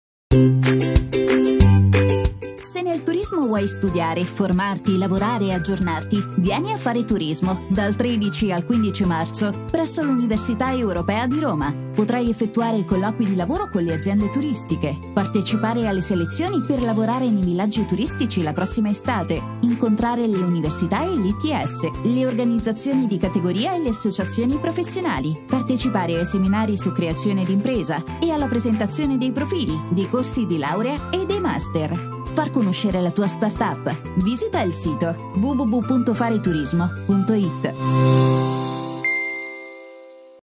FareTurismo 2019_ Università europea Roma
FareTurismo, l'unico appuntamento nazionale dedicato alla formazione, al lavoro e alle politiche turistiche, presso l'Università Europea di Roma